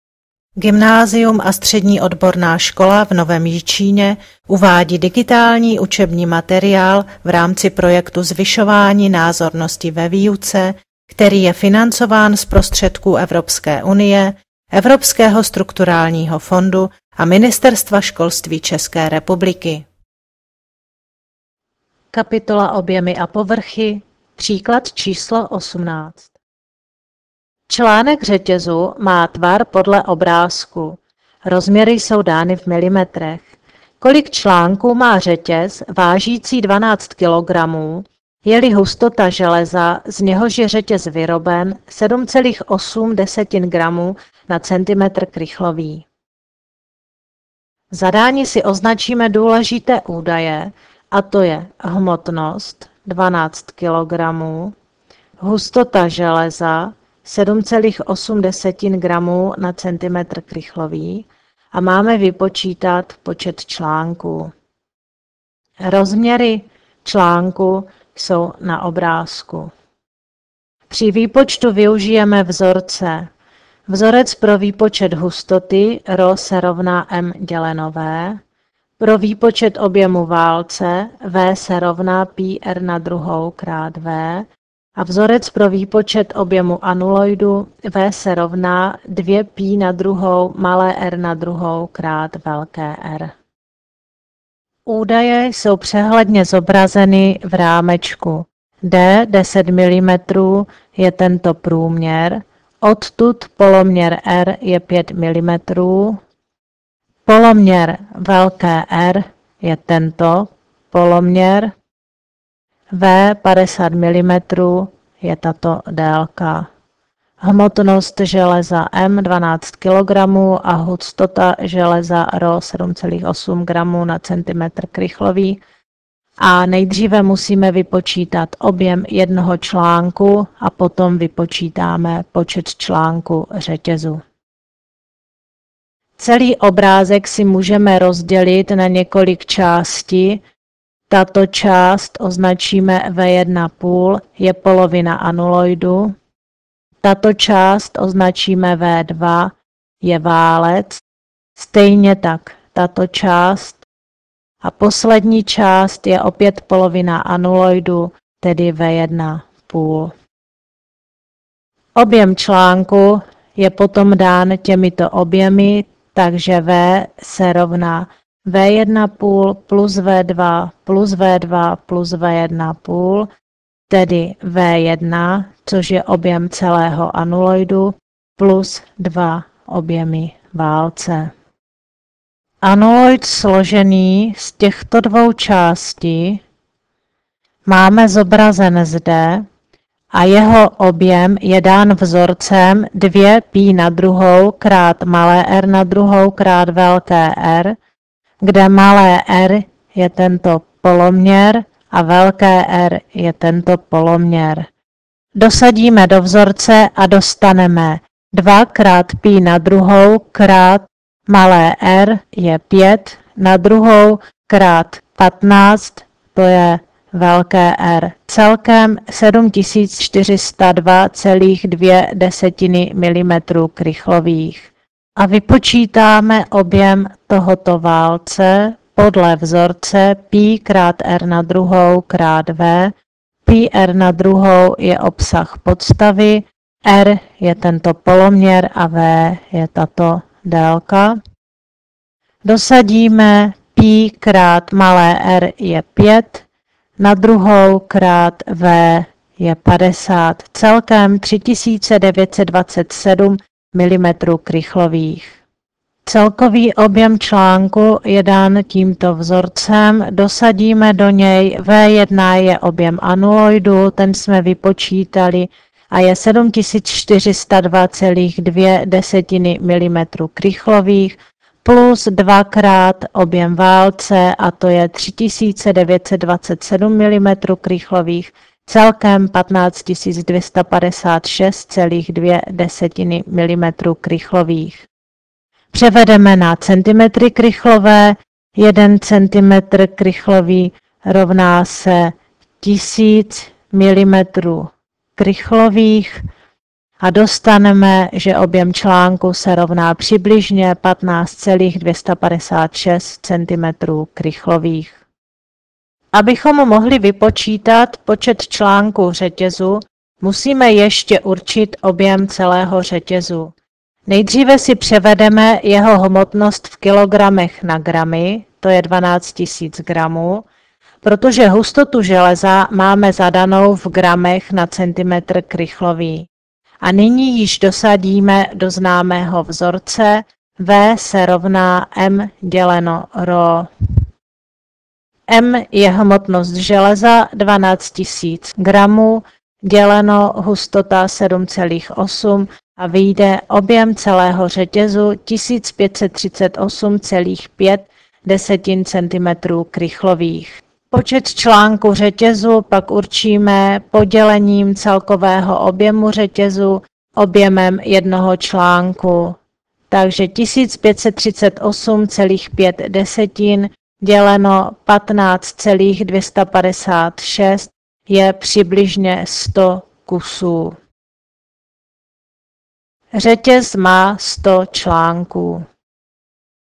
Tu18
Objem článku řetězu, objem anuloidu a válce, hustota -- řešení slovní úlohy